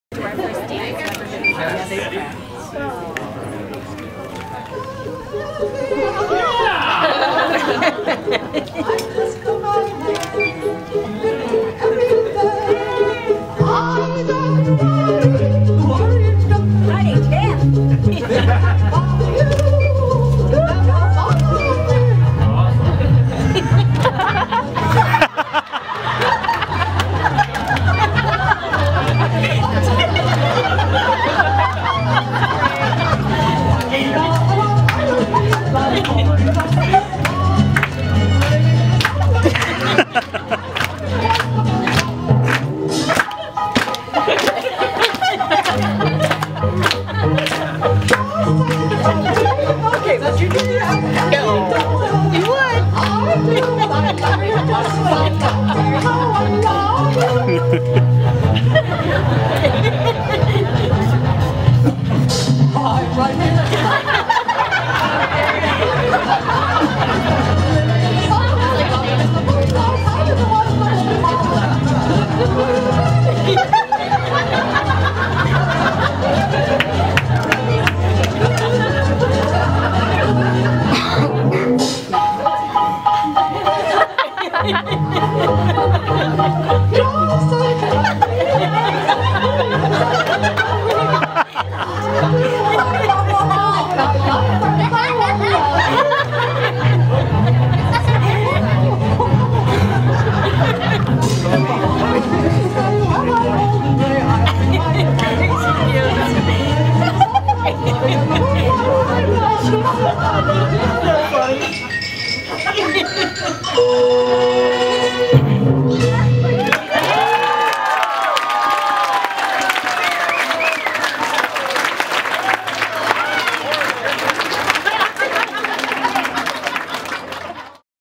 Jessie and Tanner's First Dance
Jessie and Tanner share their first dance as a married couple.